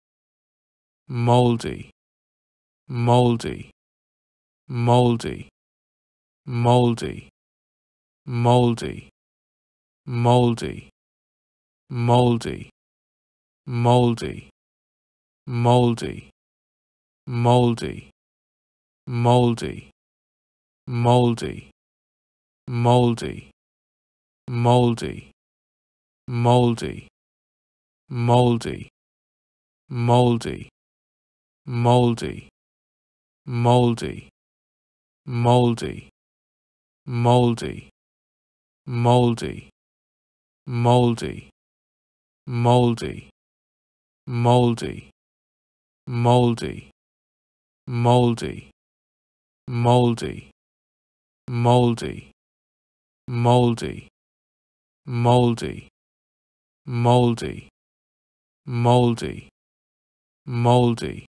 How to pronounce Mouldy||How to say Mouldy||Mouldy Pronunciation||ABDictionary
Do you want to know how to pronounce Mouldy? In this video you'll learn how to say or pronounce Mouldy for free